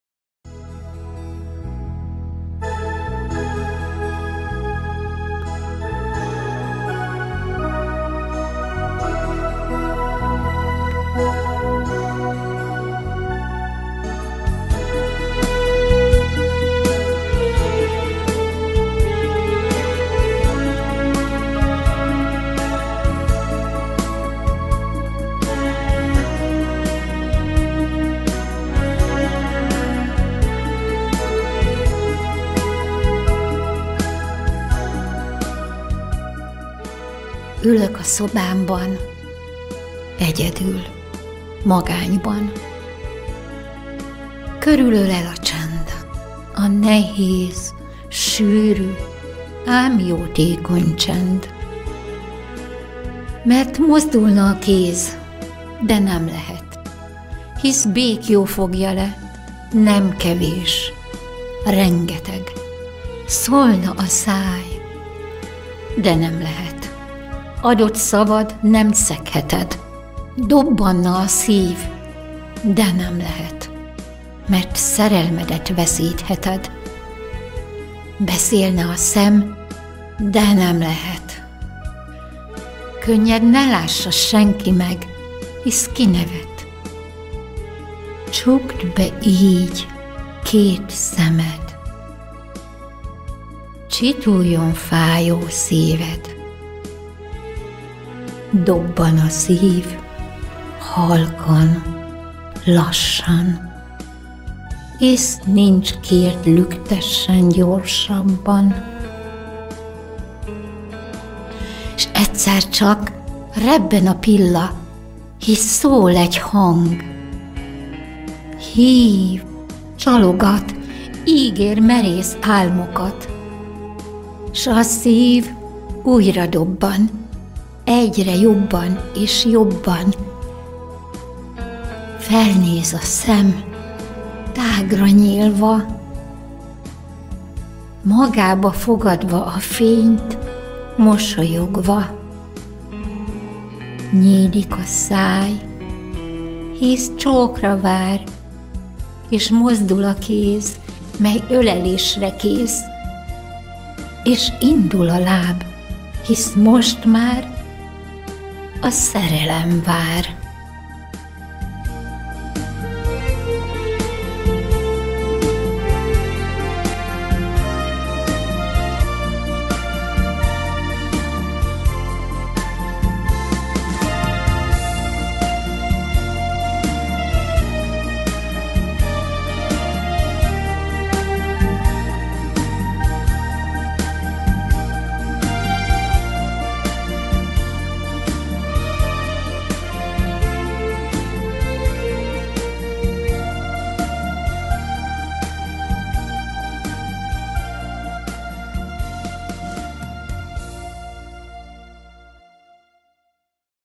A csend * Takács Lia verse
Takács Lia: A CSEND

Ülök a szobámban, egyedül - magányban..
Körülölel a csend , a nehéz, sűrű ám jótékony csend.
Mert mozdulna a kéz, de nem lehet,
hisz béklyó fogja le, nem kevés, rengeteg.
Szólna a száj, de nem lehet, adott szavad nem szegheted!
Dobbanna a szív, de nem lehet,
Mert szerelmedet veszítheted!
Beszélne a szem, de nem lehet!
Könnyed ne lássa senki meg, hisz kinevet!
Csukd be így két szemed, csituljon fájó szíved!
Dobban a szív, halkan - lassan,
Hisz nincs kiért lüktessen gyorsabban.
S egyszer csak. rebben a pilla, hisz szól egy hang!
Hív, csalogat, ígér merész álmokat!
S a szív újra dobban, egyre jobban és jobban.
Felnéz a szem, tágra nyílva,
magába fogadva a fényt mosolyogva.
Nyílik a száj, hisz csókra vár és mozdul a kéz,
mely ölelésre kész!
És indul a láb, hisz most már a
Szerelem vár!